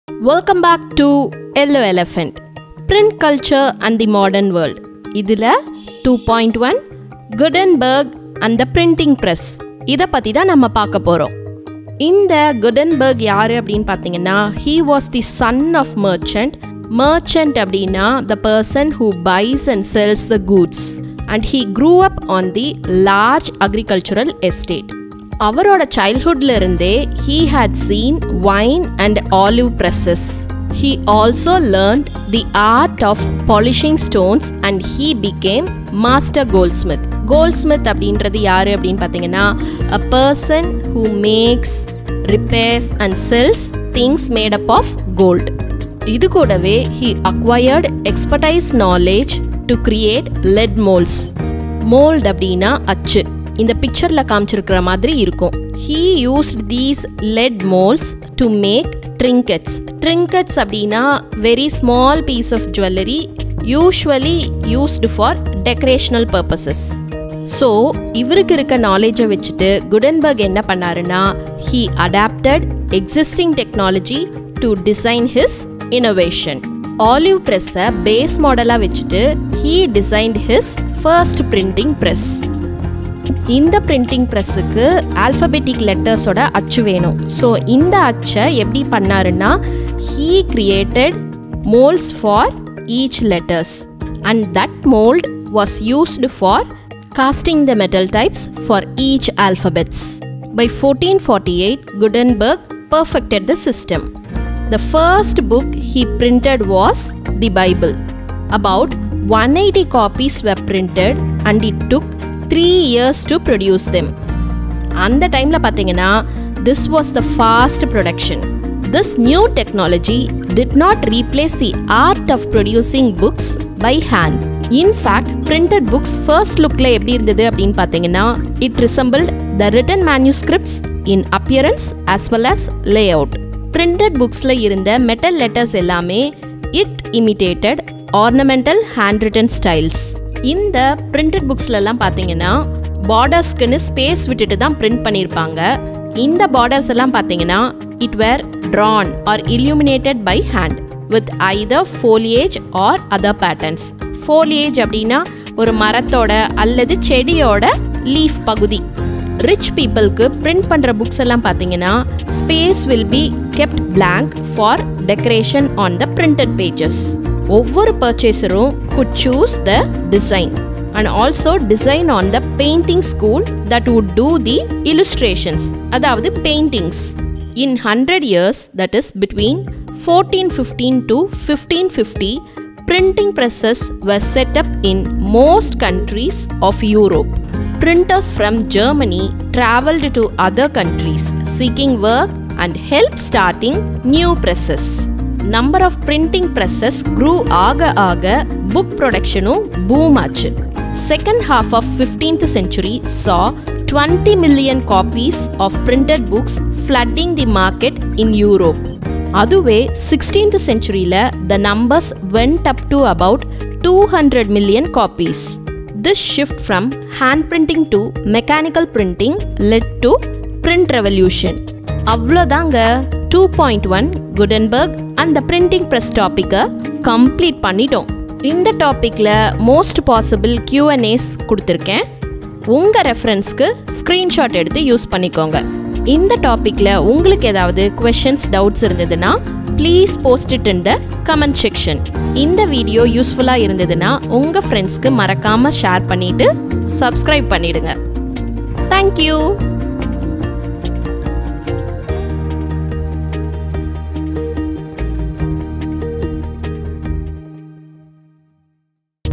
CBSE X History - Print Culture and the Modern World - 2.1 Gutenberg and the printing press - Tamil.
Hello Viewers,

This video explains in Tamil about
CBSE X History - Print Culture and the Modern World - 2.1 Gutenberg and the printing press.

1) How did Gutenberg personalise the printed books suiting to the tastes and requirement of others [CBSE 2012].
2) Describe any three main features of the first printed Bible. [CBSE 2014]
3) ‘The shift from hand printing to mechanical printing led to the print revolution.’ Explain.

Lots of Gratitude!